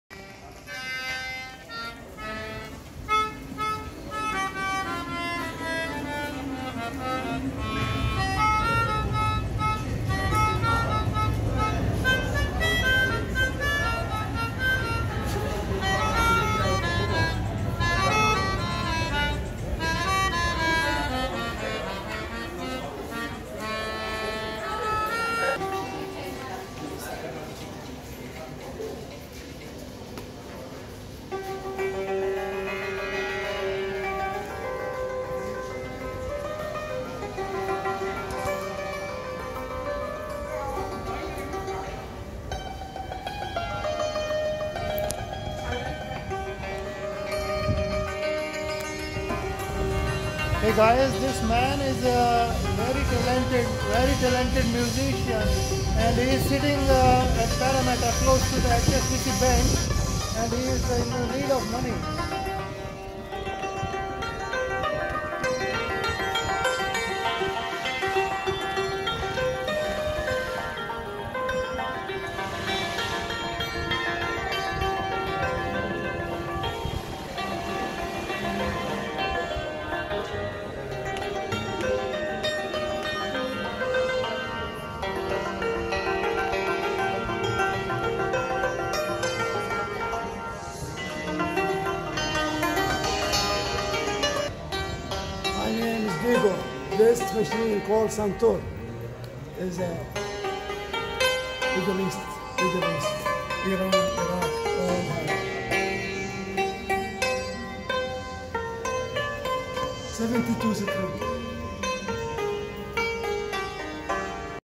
This Suntor player in Parramatta is so talented but need money to support
This Suntor player in Parramatta is so talented but need money to support. Is Bollywood or any music company listening?